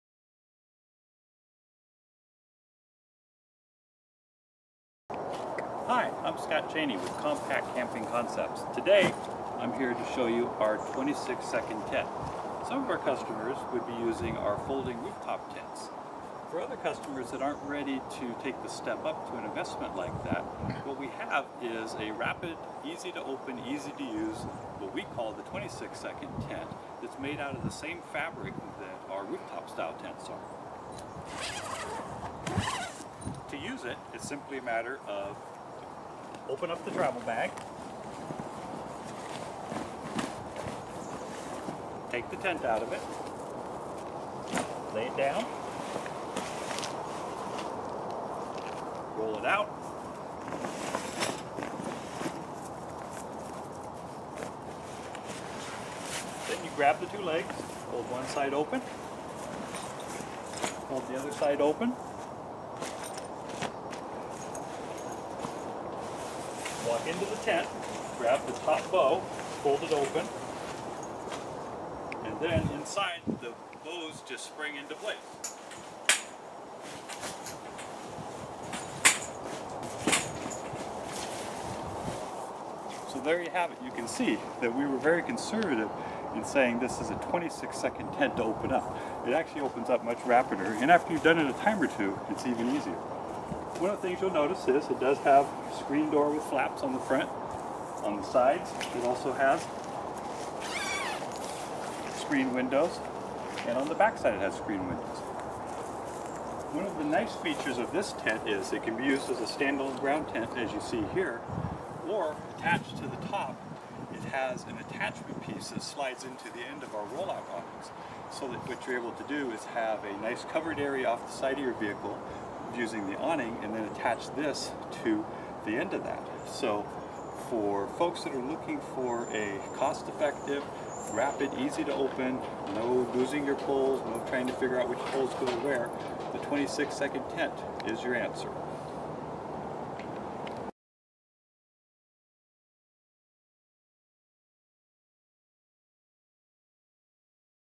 26_Second_Tent
26 Second Tent from Compact Camping Concepts much like the OzTent - 30 Second Tent; but faster!  See how easy it is to set up and get over to order!